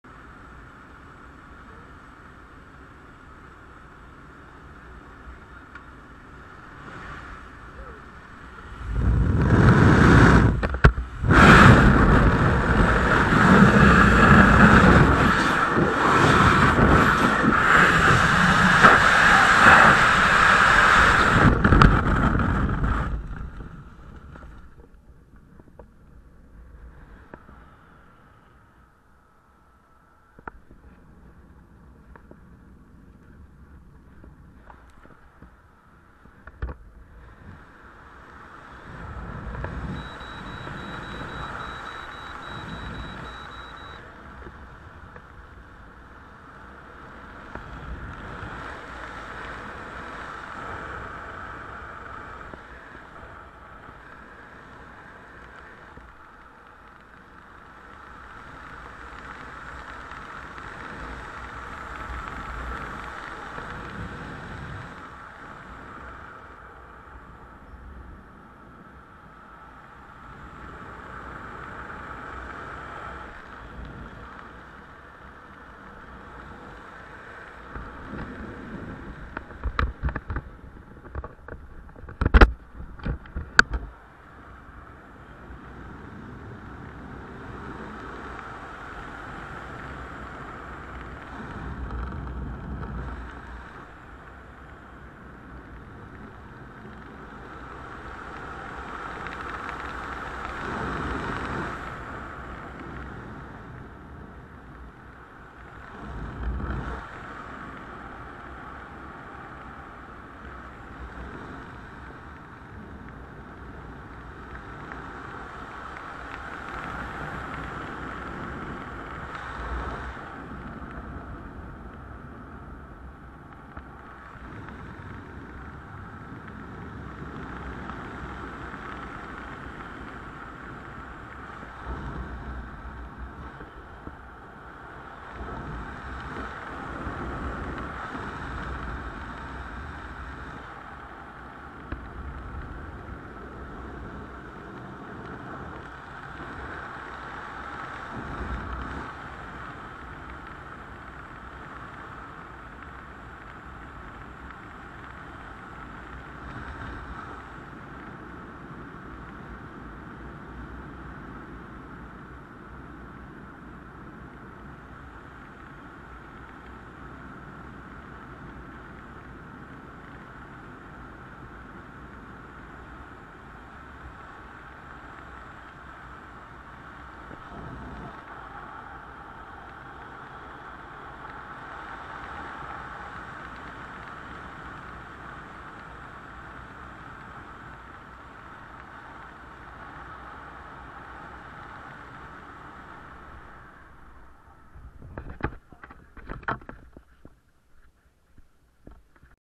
Skydiving Accuracy landing (Intermediate)
Skydiving Accuracy jump, from 5000 feet, at Skydive Swansea, Wales, UK. Target zone is 5M with a 16cm plate in the middle. Youl have to watch untill the end to see if i land in the target or not ;)